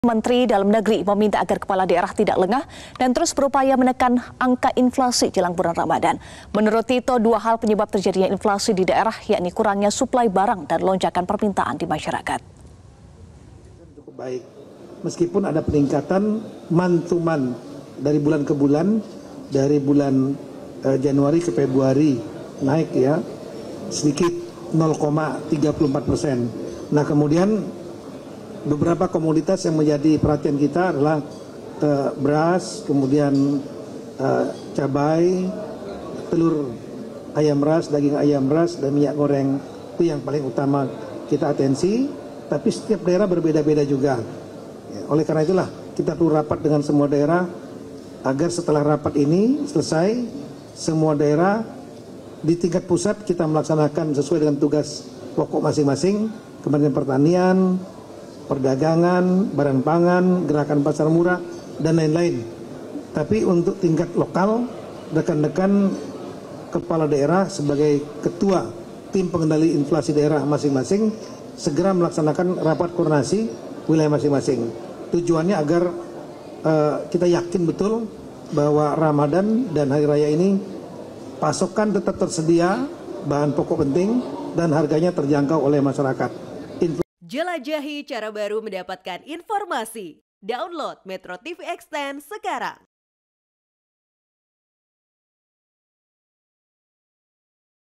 Mendagri Minta Kepala Daerah Antisipasi Inflasi Jelang Ramadan
MetroTV, Mendagri  Tito Karnavian meminta gubernur, bupati dan wali kota di Tanah Air untuk mengantisipasi inflasi menjelang masuknya Bulan Suci Ramadhan 1445 Hijriah.